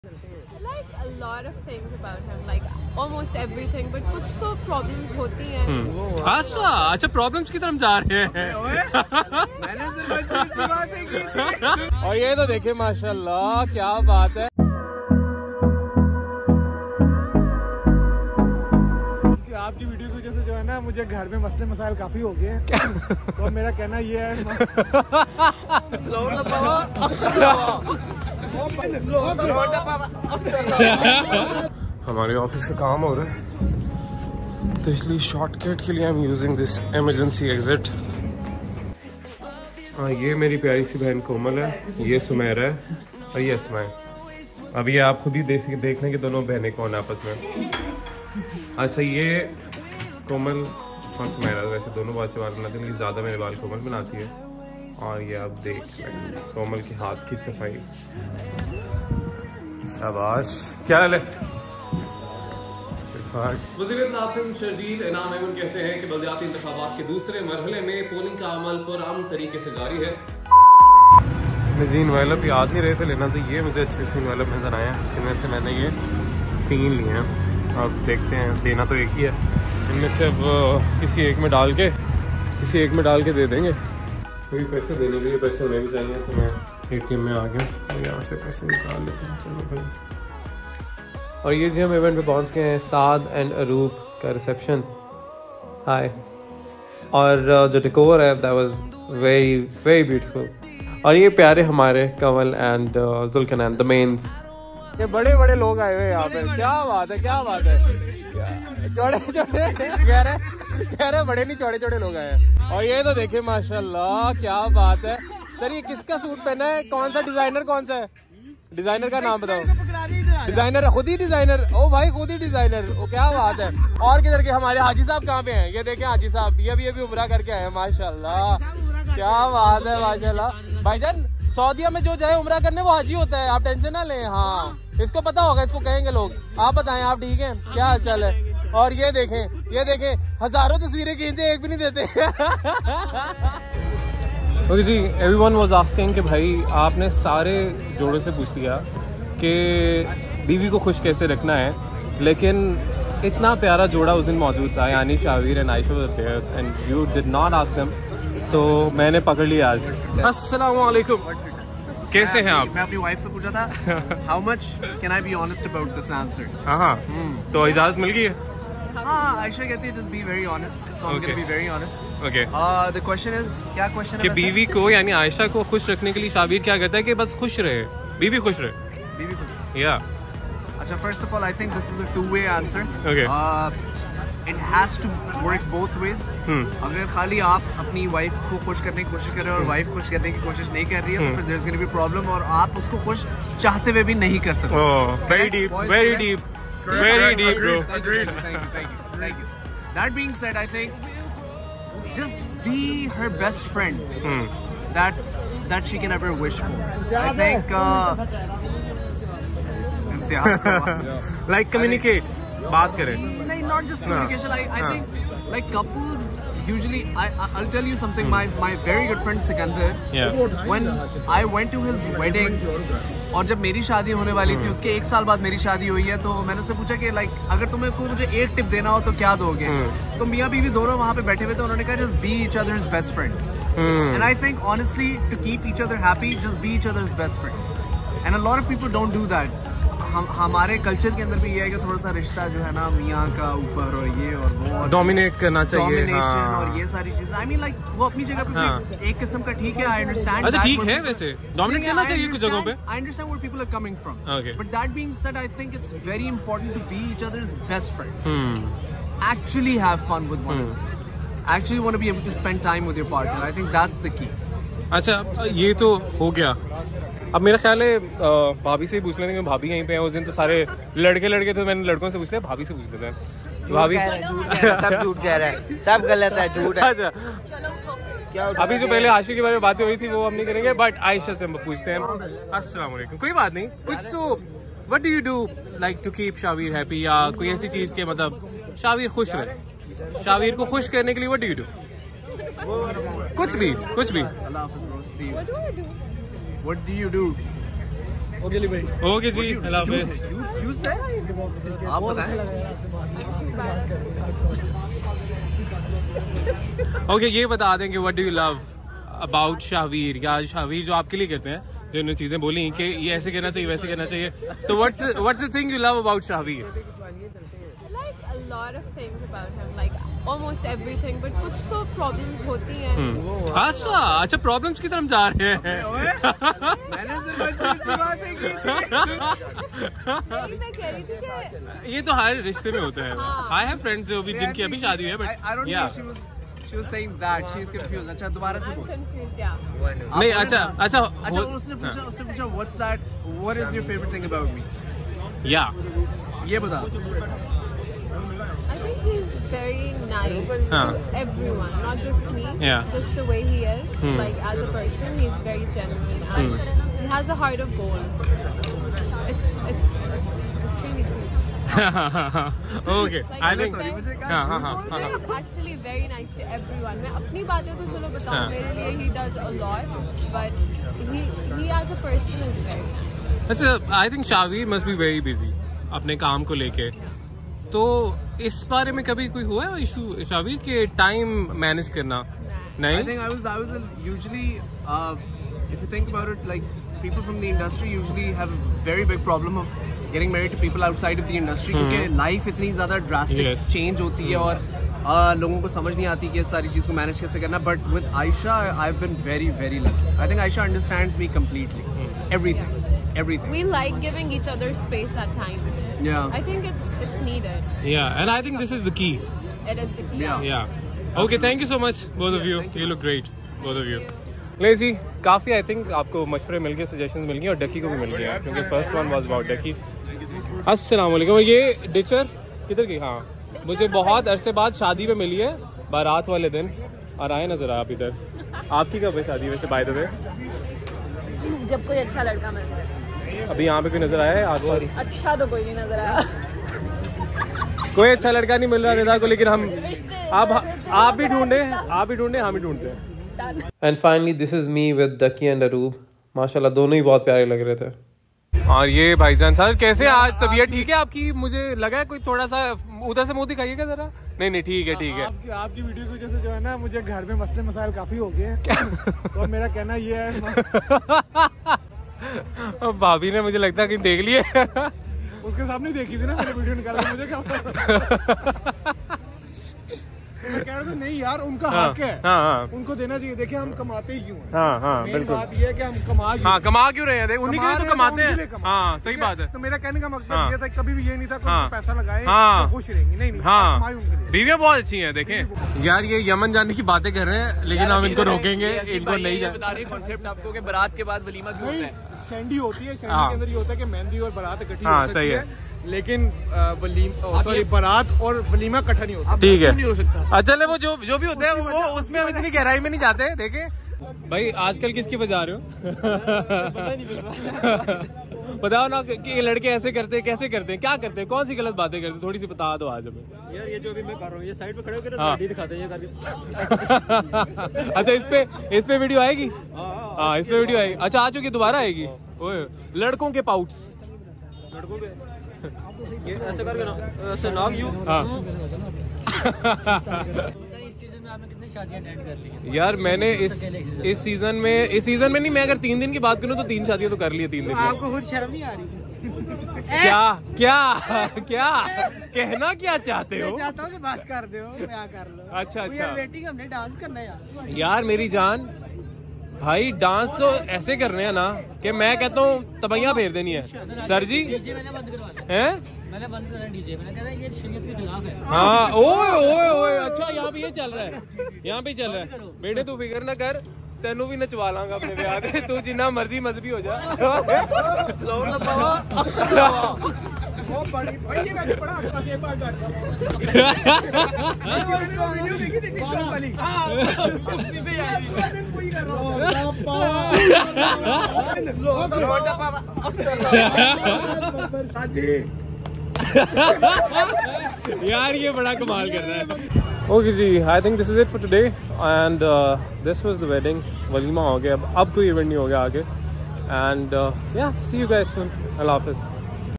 RELATIONSHIP GOALS FT. SHAHVEER & AYESHA | DUCKY KI SHADI
Attended Ducky's Walima where I met this beautiful couple Shahveer & Ayesha, loved their chemistry ❤️